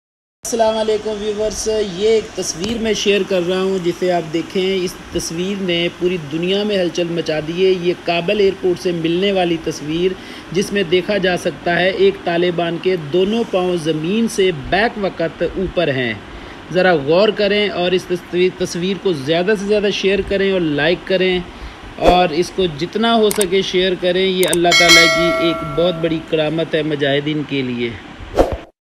A photo from Kabul Airport that shook the world that Mujahid's
A photo from Kabul Airport that shook the world that Mujahid's two feet are above the ground at the same time